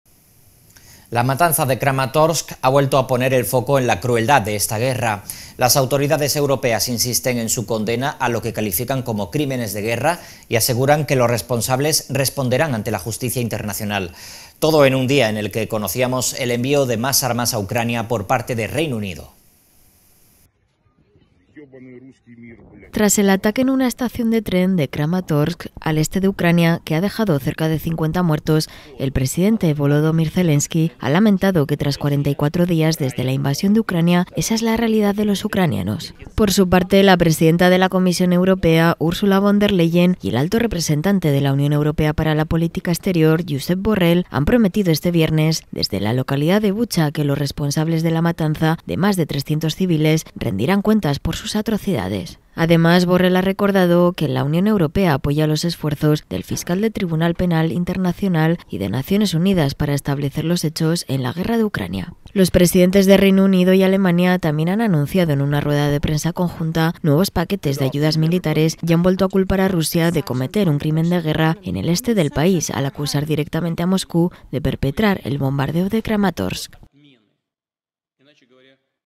🔴Noticia - Europa, conmocionada tras la matanza de Kramatorsk
Tras el ataque en una estación de tren de Kramatorsk, al este de Ucrania, que ha dejado cerca de 50 muertos, el presidente Volodomir Zelenski ha lamentado que, tras 44 días desde la invasión de Ucrania, "esta es la realidad" de los ucranianos. (Fuente: EBS / 10 downing Street / Gobierno de Ucrania)

English:

After the attack on a train station in Kramatorsk, in eastern Ukraine, which has left nearly 50 dead, President Volodomir Zelensky lamented that, after 44 days since the invasion of Ukraine, "this is the reality" of the Ukrainians . (Source: EBS / 10 downing Street / Government of Ukraine)

Live streaming